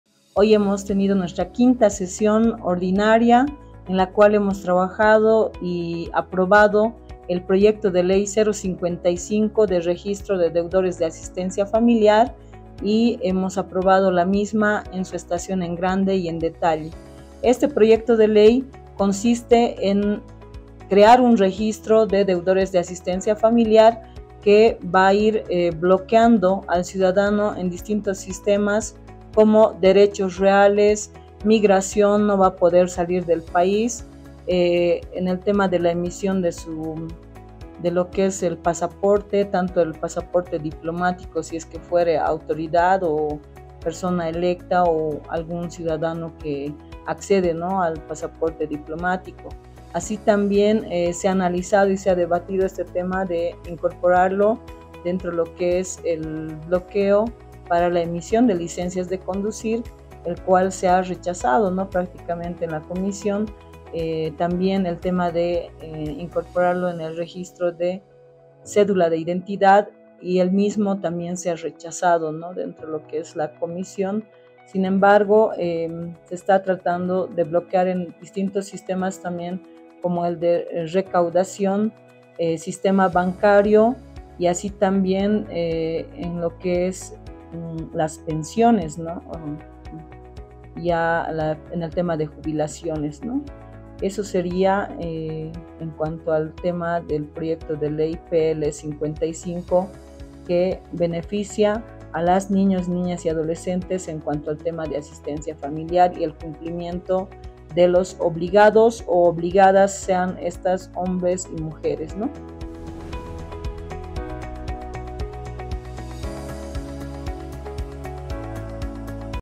31/01/2024 La Comisión de Derechos Humanos, aprobó en su Sesión Ordinaria, el Proyecto de Ley N° 055
La Comisión de Derechos Humanos, aprobó en su 5ta Sesión Ordinaria, el Proyecto de Ley N° 055 “De Registro de Deudores de Asistencia Familiar”, que busca crear un registro de deudores de asistencia familiar que ira bloqueando en distintos sistemas como; derechos reales, migración y la emisión de pasaje diplomático.